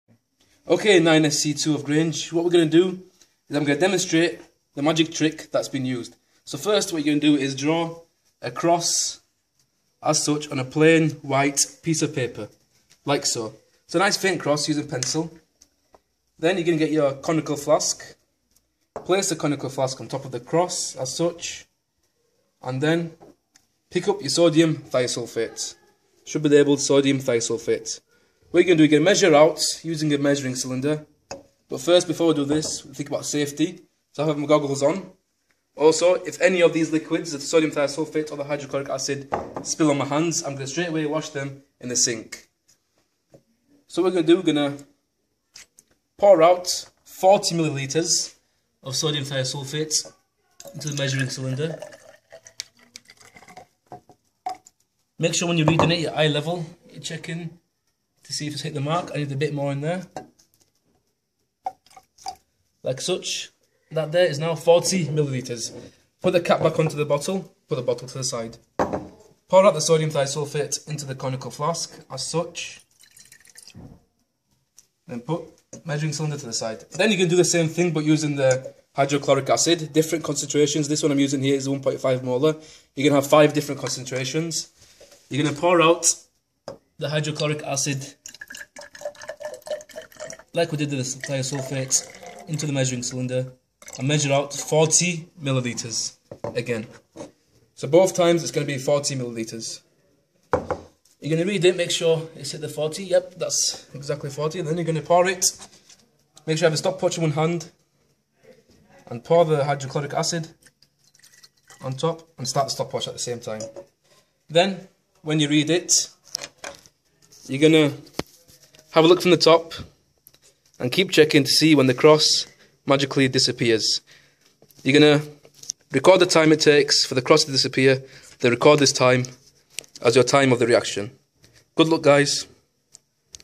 Sodium thiosulphate disappearing cross reaction
Disappearing cross reaction - sodium thiosulphate and hydrochloric acid.